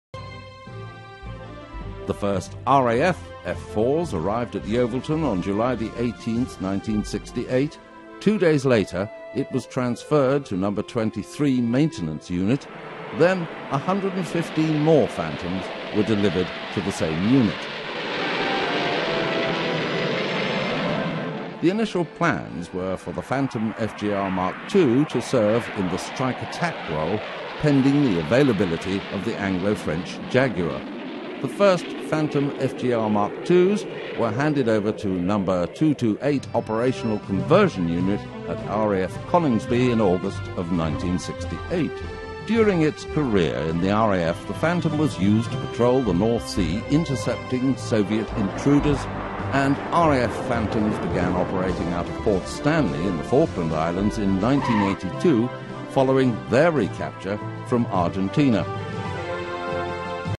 RAF Phantom FGR.2
The United Kingdom operated the McDonnell Douglas F-4 Phantom II as one of its principal combat aircraft from the 1960s to the early 1990s.

Although assembled in the United States, the UK's Phantoms were a special batch built separately and containing a significant amount of British technology; the F-4M version was procured for the RAF to serve in the tactical strike and reconnaissance roles.